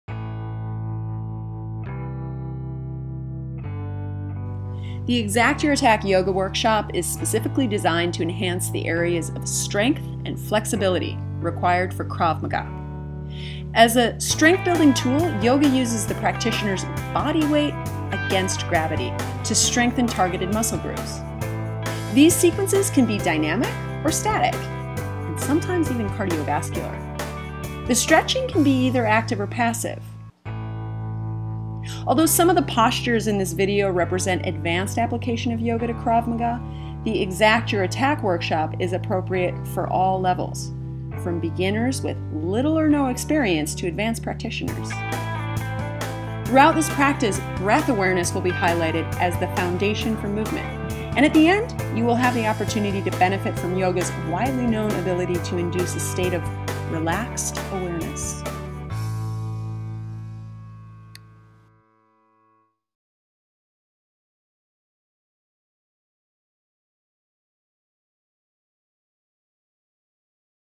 Krav Maga and Yoga - Exact Your Attack Yoga for Krav. Krav Maga NYC
As a strength building tool, yoga uses the practitioner's body weight
against gravity to strengthen targeted muscle groups.

These sequences can be dynamic or static and sometimes even cardiovascular.

The stretching can be either active or passive.

Although some of the postures in this video represent advanced
application of yoga to Krav Maga, the Exact Your Attack workshop is
appropriate for all levels. From beginners with little or no
experience to advance practitioners.
First of its kind!

Throughout this practice, breath awareness will be highlighted as the
foundation for movement and at the end, you will have the opportunity
to benefit from yoga's widely known ability to induce a state of
relaxed awareness.